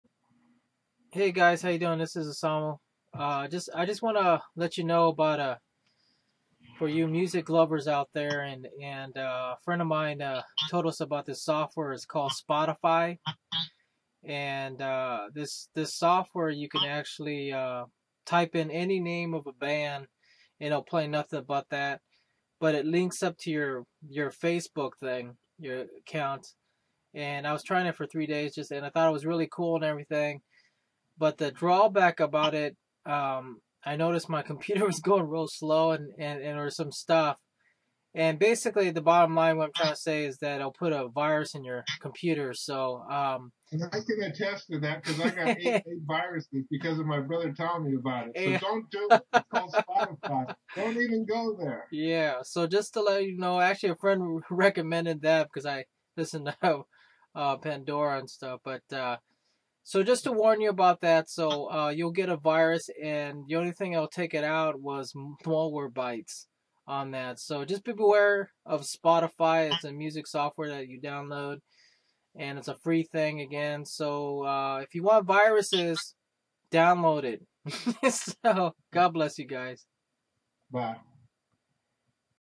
Beware of Spotify! Music Virus
Beware of Spotify Music software brings Viruses on your computer.